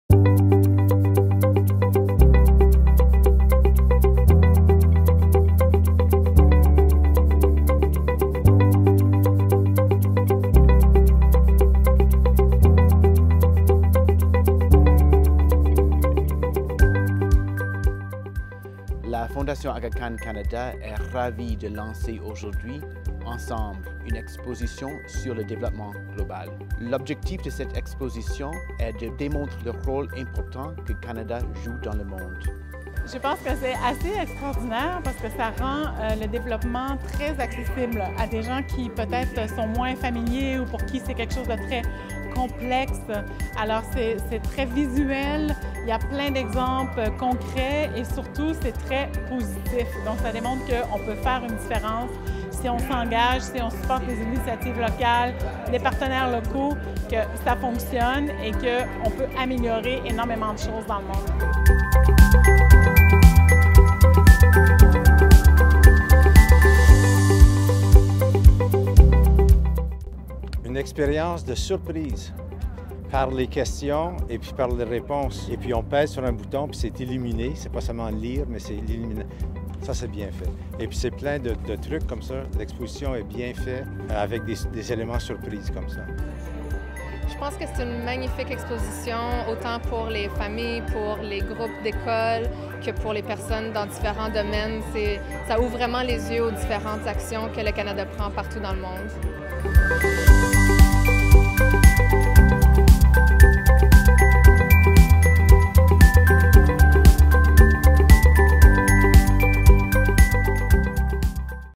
Ensemble : une exposition sur le développement mondial
Ensemble : une exposition sur le développement mondial prend la route pour visiter des communautés dans l’ensemble du pays afin d’explorer comment les Canadiens et les Canadiennes dirigent le changement à l’échelle mondiale.

Passez le mot!